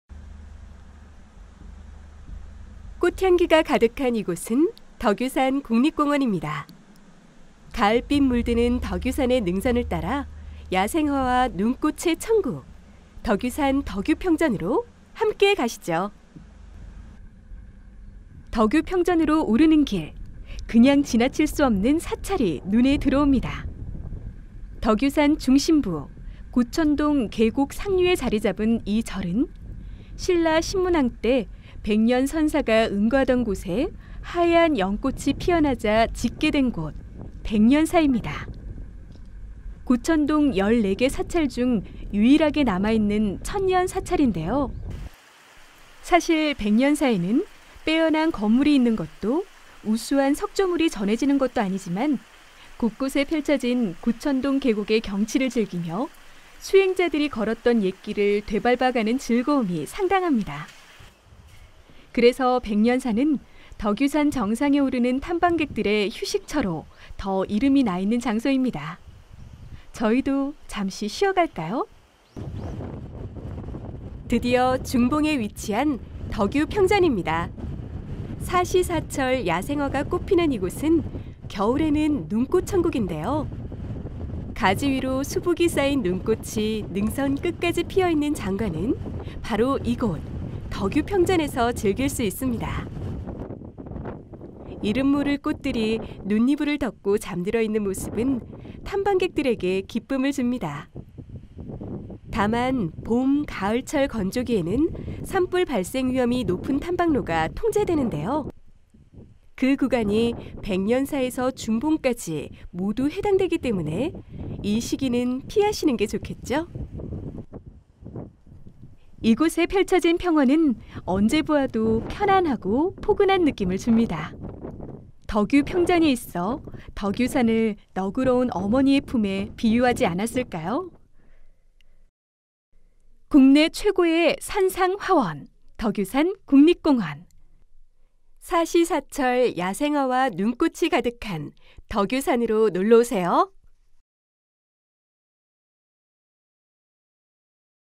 [4K] VR(360도 영상)로 즐기는 덕유산국립공원 가상탐방 / 덕유평전 / Deogyusan National Park /
VR(360도 영상)로 즐기는 덕유산국립공원 가상탐방 / 덕유평전 / Deogyusan National Park /

백두대간의 중심,  한국의 알프스 아고산대 생태계의 보존가치가 높은 덕유산!

이제 집에서도 덕유산국립공원을 만나보세요.

Virtual tour of Deogyusan National Park

Discover the beautiful trails and nature of the Deogyusan National Park.

Deogyusan National Park is called “The Heaven of Wild Flowers.” As for animals, there are 32 mammal species, 130 bird species, 9 amphibian species, 13 reptile species, 28 fish species, and 1,337 insect species inhabiting the area. Deogyusan is rich in biodiversity with 2,039 species altogether, and animals such as the flying squirrel, Otter, Marten cat, and Small-eared cat have been designated as endangered species by the Ministry of Environment.